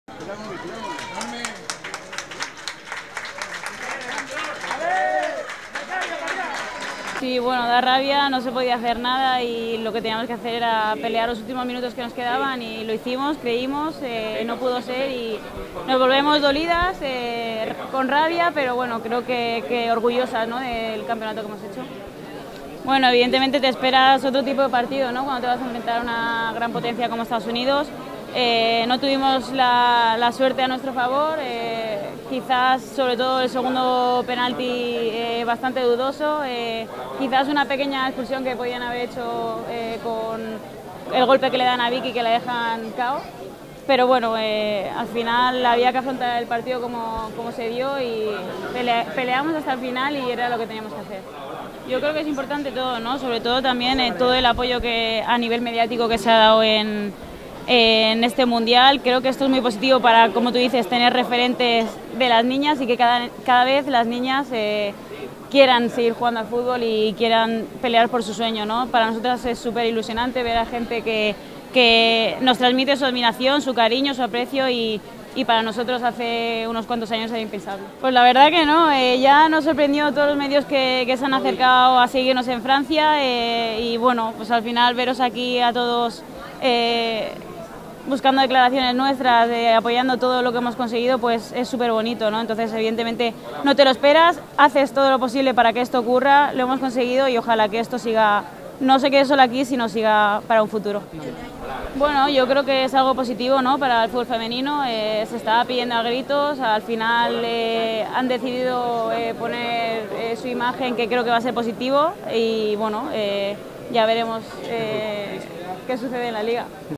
Sandra Paños: "Nos volvemos dolidas pero orgullosas" | Diario AS
La guardameta de la Selección habló de la derrota ante Estados Unidos y del papel de España en el Mundial a su llegada a Madrid. Disfruta de la mejor actualidad deportiva en nuestra web!